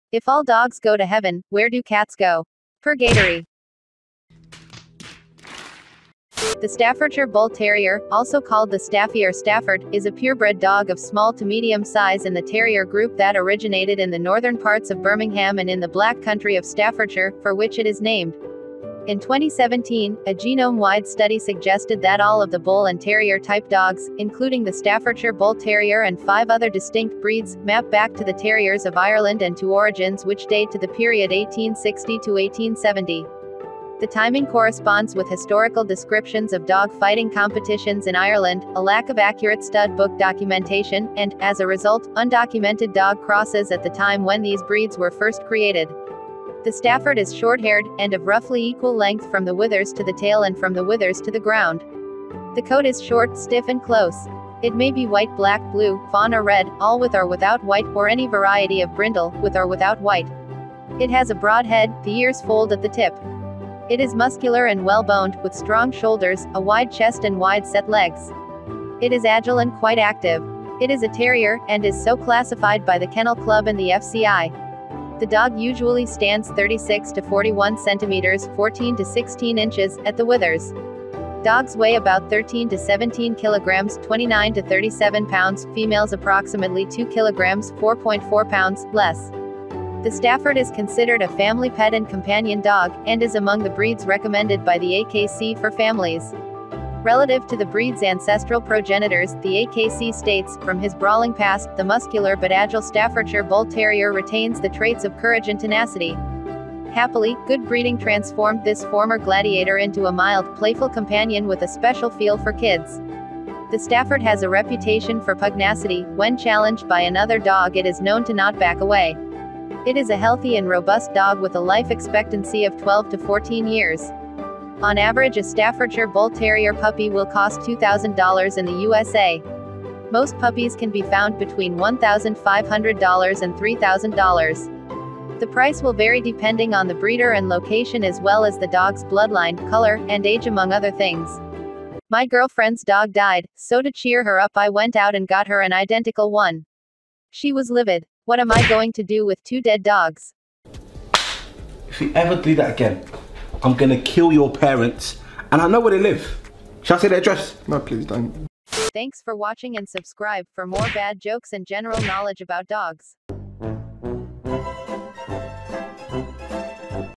Quick Dog Facts About The Staffordshire Bull Terrier!
In today's video we're gonna see some quick dog facts about the Staffordshire Bull Terrier.